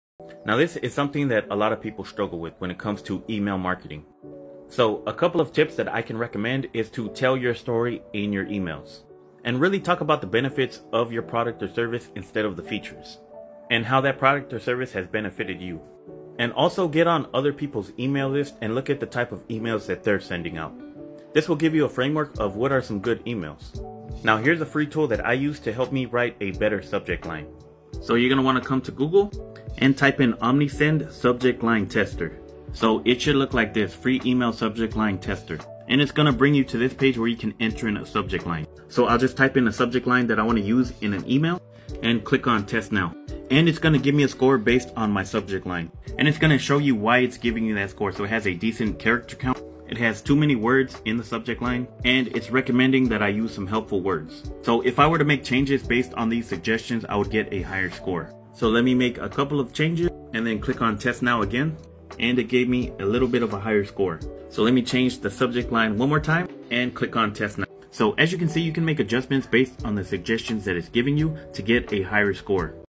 Tips For Email Marketing
Writing out an email campaign where you're telling your story is the best way to connect with your audience.

Tool I Use For Youtube:🔻
Sony ZV-1 Camera
Rode Wireless Go 2 Microphone